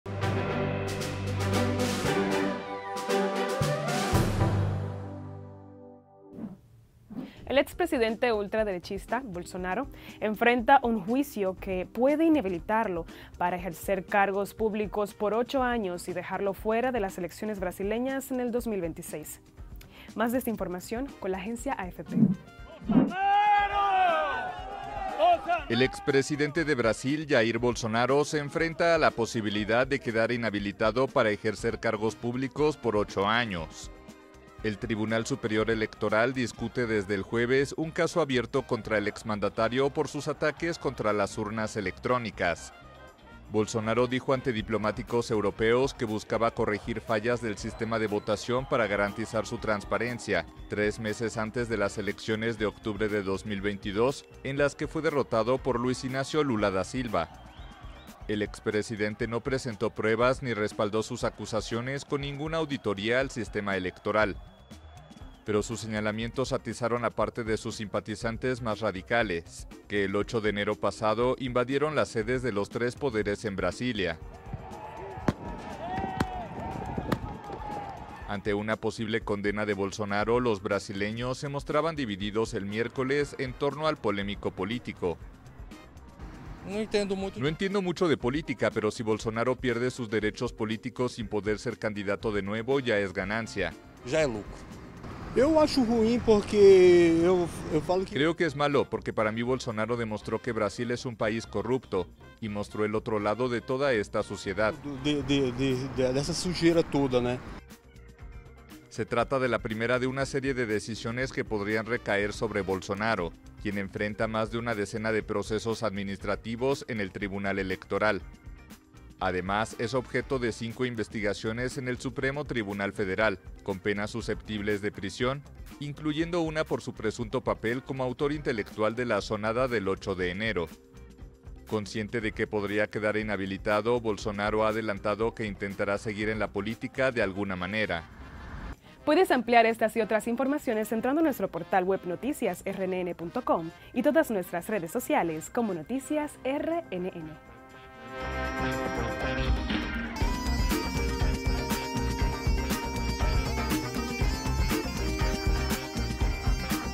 Bolsonaro enfrenta juicio que puede inhabilitarlo para próximas elecciones
El expresidente ultraderechista Jair Bolsonaro enfrenta un juicio que puede inhabilitarlo para ejercer cargos públicos por ocho años y dejarlo fuera de las elecciones brasileñas de 2026.